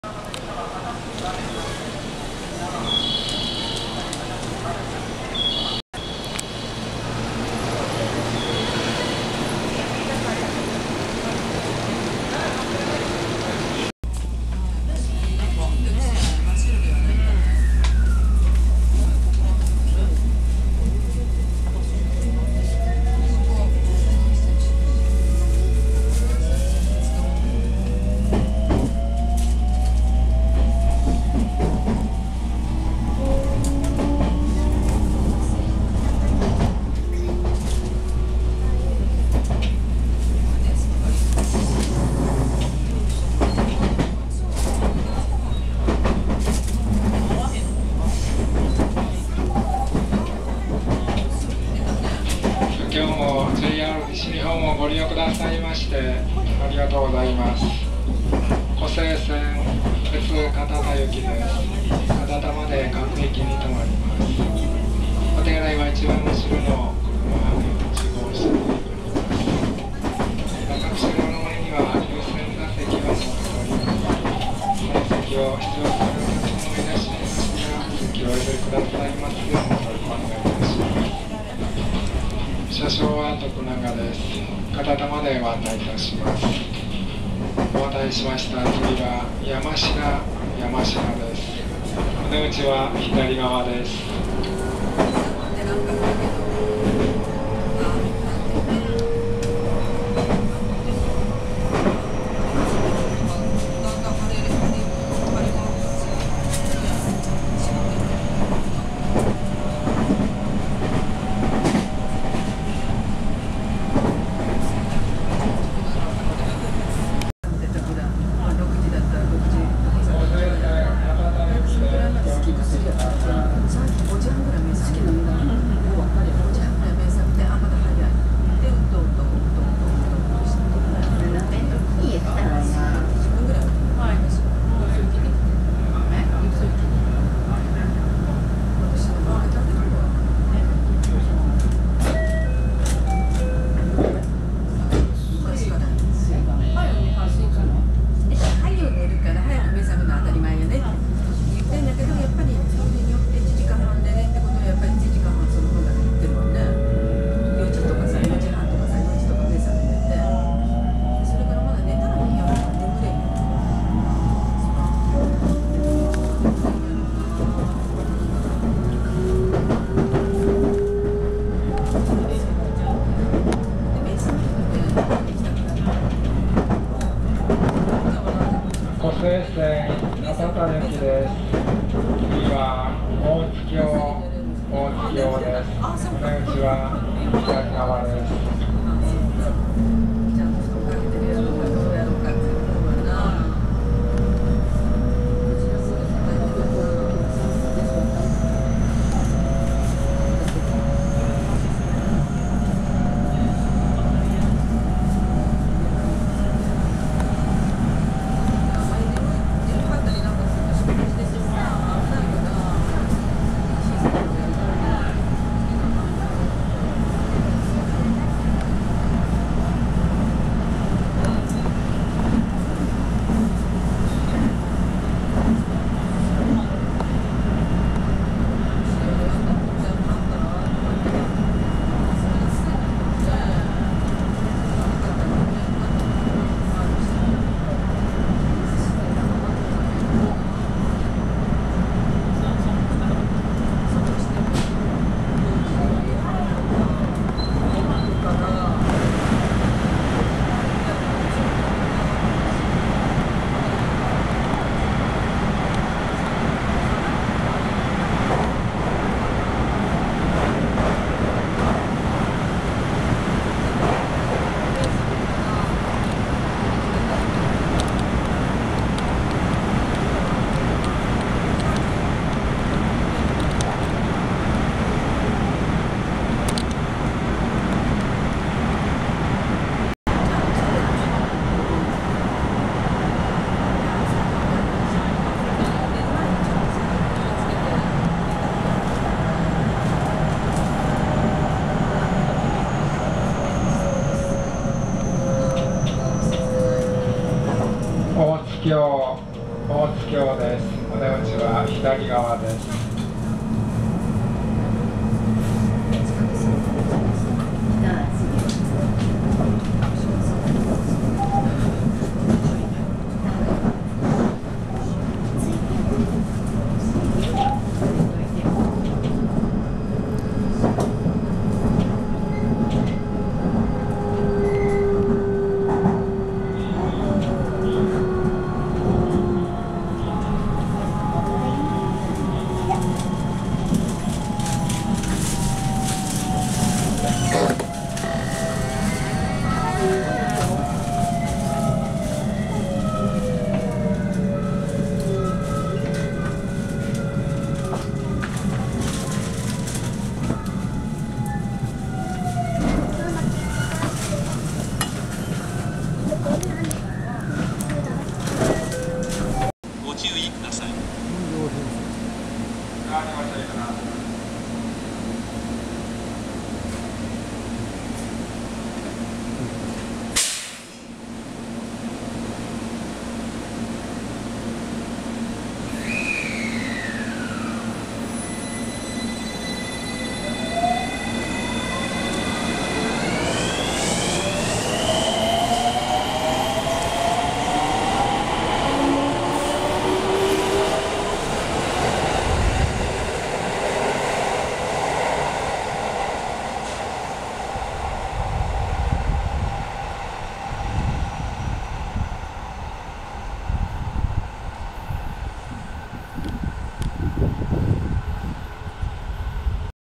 JR西日本 223系2000番台 V56編成 B普通 堅田行き 湖西線運用 京都駅 唐崎駅 20190715
JR西日本 223系2000番台 V56編成 B普通 堅田行き 湖西線運用
京都駅  唐崎駅